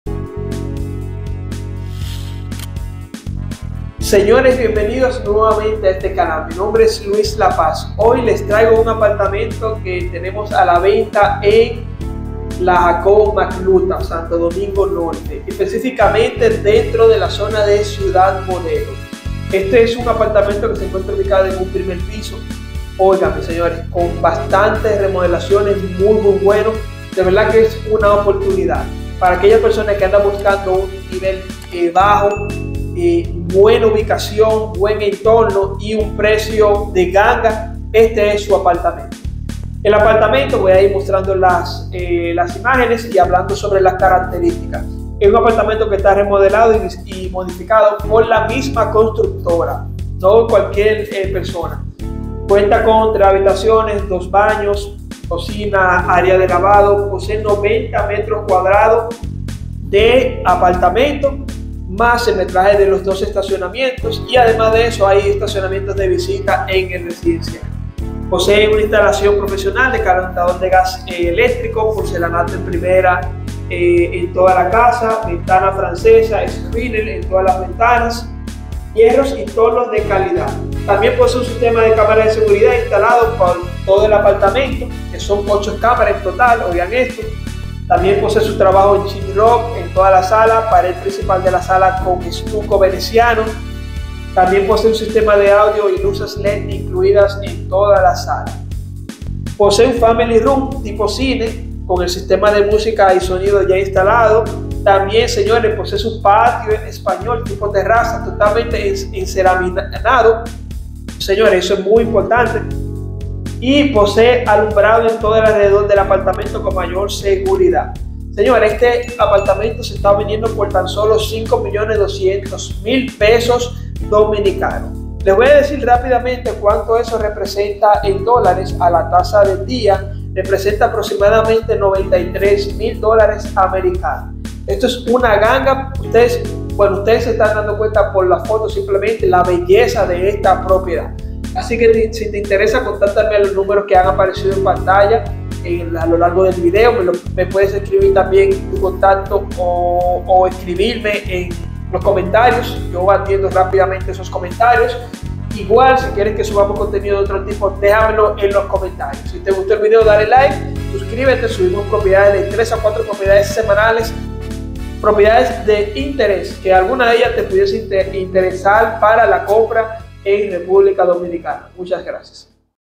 Apartamento REMODELADO con TERRAZA en Santo Domingo Norte | Jacobo Majluta | Primer Piso
Hola, Mi nombre es Luis M. La Paz, Asesor inmobiliario de Hogarium Real Estate, República Dominicana. Estoy colocando a tu disposición más de 8 años de experiencia en el sector inmobiliario.

A continuación, las informaciones del inmueble del video:

Apartamento en venta en la Jacobo Majluta, Ciudad Modelo Norte 1er Piso

Características

-Esta remodelado y modificado por la misma contructora  y cuenta con lo siguiente:

3 habitaciones
2 baños
Cocina
Area de Lavado
90mt2 propiedades + 2 parqueos+parqueos de visitas.
-Intalacion profesional de calentador  a gas y eléctrico.
-Poncelanato de primera en toda la casa.
-Ventanas francesada
-Screener en todas las ventanas.
-Hierros y toldos de calidad.
-Sistemas de cámara de seguridad intalada por todo el apartamento.
(8 cámaras).
-Trabajo en Sheetrock en toda la sala.
-Pared principal de la sala en arte de estucos venecianos.
-Sistema de audio y luces led incluidas en toda la sala.

-Family ROOM tipo cine con el sistema de música y sonido instalado.

-Patio español tipo terraza totalmente enceramicado.

-Alumbrado en todo el alrededor del apartamento para mayor seguridad.
-Diseño de arbolado en todo el alrededor del apartamento.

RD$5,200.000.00